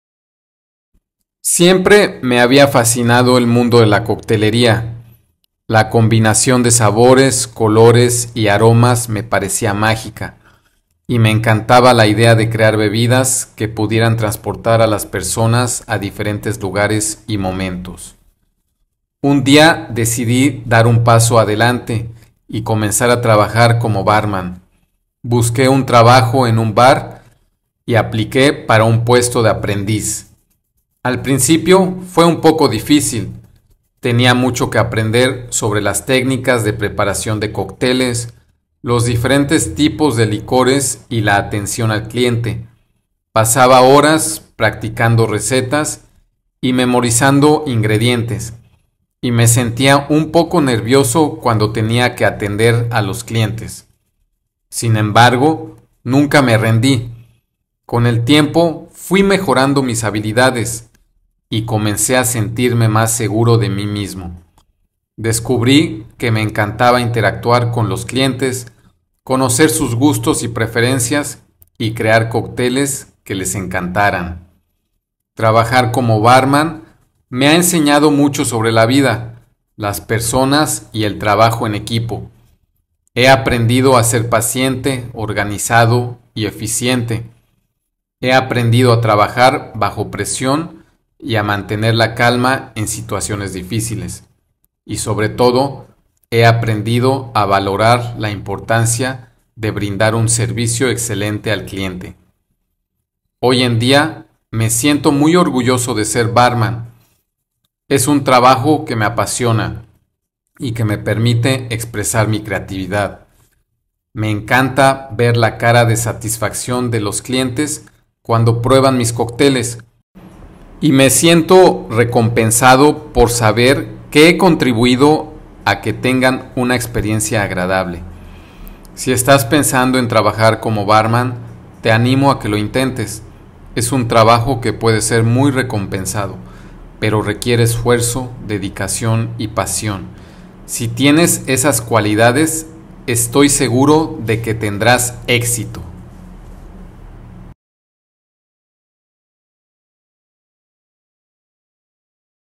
Spanish Learning Through Stories - My life as a Barman (A2-B2)
¡Brindando por el aprendizaje! Mi vida como bartender (Comprensión auditiva)

¿Alguna vez te has preguntado qué sucede detrás de la barra? En este video, escucharás mi historia como bartender. A través de anécdotas y experiencias, te sumergirás en el mundo de la coctelería y la atención al cliente en un bar.

⭐Follow us along with our Spanish learners